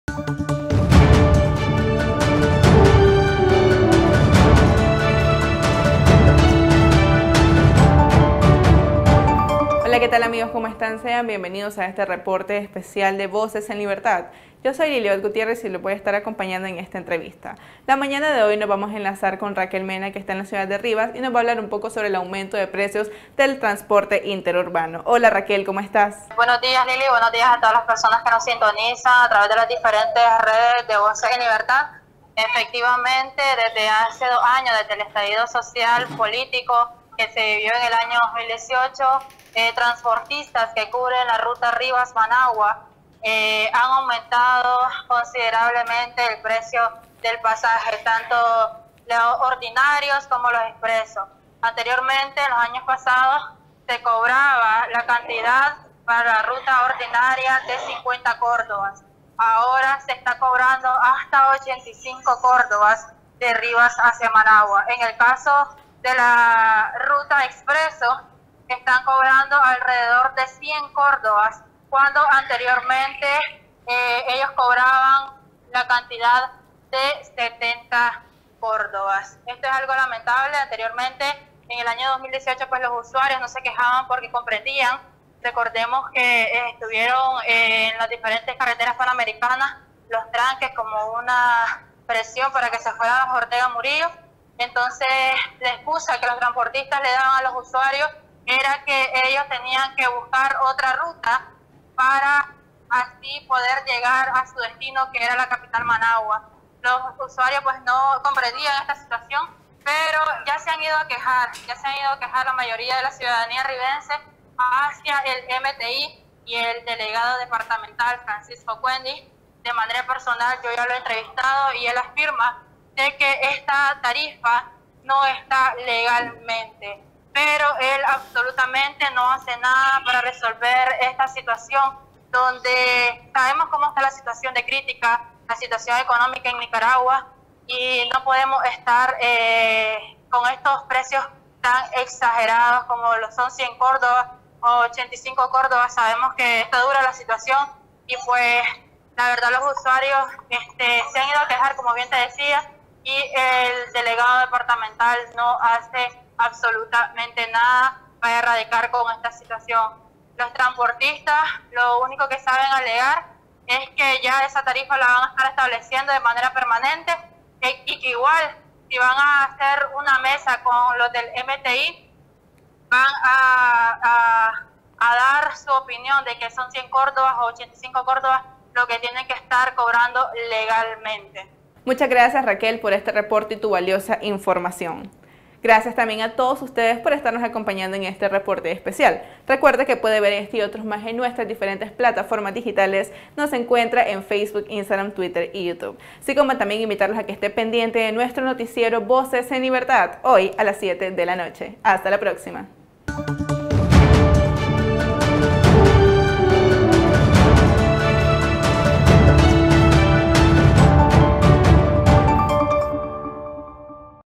REPORTE ESPECIAL
El costo del pasaje interurbano en Rivas ha venido incrementándose en los dos últimos años sin que las autoridades expliquen las razones. Detalles en el reporte de nuestra corresponsal Raquel Mena.